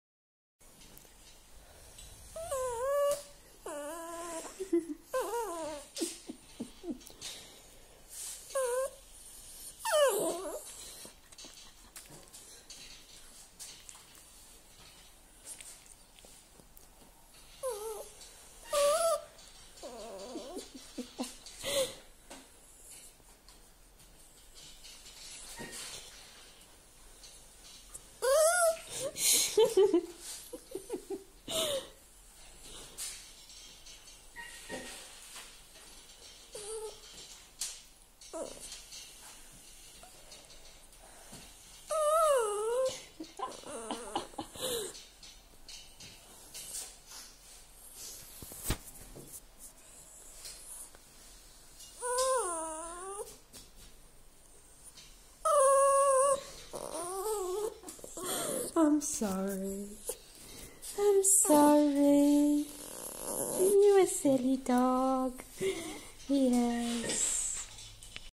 Dog Moans When Blown On
My nans dog, Mia, moaning at me whilst I blow on her head!